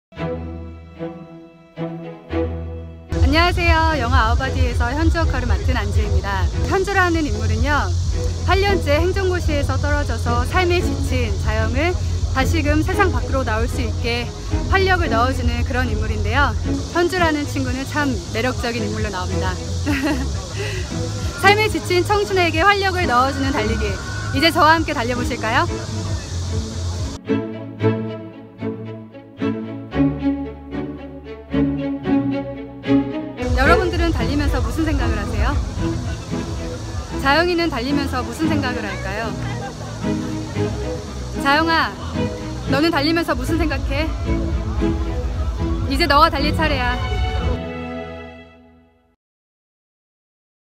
달리는 청춘 현주 역 배우 안지혜, 영화 개봉 앞두고 청춘 달리기 응원 영상
오는 9월 26일 개봉을 앞두고 있는 영화 [아워 바디]에서 달릴 때 느껴지는 건강한 활력으로 주인공 자영의 시선을 사로잡으며 함께 달리는 여자 ‘현주’ 역을 섬세하게 연기해 주목받고 있는 배우 안지혜가 청춘 응원 영상을 공개해 화제다. 안지혜의 깊이 있는 연기가 돋보이는 영화 [아워 바디]는 8년간 행정고시에 번번이 떨어지며 몸과 마음이 지쳐버린 자영이 우연히 달리는 여자 현주를 만나 달리기 시작하면서 삶의 변화를 맞이하는 이야기를 섬세하게 접근한 작품이다.

배우 안지혜는 지난 9/1(일) 열렸던 ‘아디다스 MBC+ 마이런 서울’에 참가하여 마라톤 10km 코스를 완주하며 화제가 된 바 있다. 현주 캐릭터에 관한 매력 넘치는 소개와 “삶에 지친 청춘에게 활력을 넣어주는 달리기, 이제 저와 함께 달려보실까요?”와 같이 방황하는 청춘을 응원하는 메시지부터, 주인공 자영 역의 최희서 배우에게 바통을 넘기는 멘트까지 담긴 영상에서는 행사 당일 마라톤 현장의 생생한 현장감과 활기 넘치는 에너지가 느껴져 보는 이들의 관심을 집중시킨다. 특히, 자영에게 “이제 너가 달릴 차례야”라는 멘트가 담겨, 추후에 진행될 ‘배우 최희서와 함께 하는 한강 러닝 이벤트’에 대한 궁금증과 호기심을 자아낸다.

달리는 청춘 현주 역의 배우 안지혜가 청춘의 달리기를 응원하는 영상을 공개해 화제를 모으고 있는 영화 [아워 바디]는 9월 26일 개봉해 관객들과 만날 예정이다.

Information

제목 : 아워 바디(Our Body)
제공 : 영화진흥위원회
배급 : ㈜영화사 진진
제작 : 한국영화아카데미(KAFA)
감독 : 한가람
출연 : 최희서, 안지혜, 이재인 외
장르 : 터닝 포인트 드라마
러닝타임 : 95분
관람등급 : 15세 이상 관람가
개봉일 : 2019년 9월 26일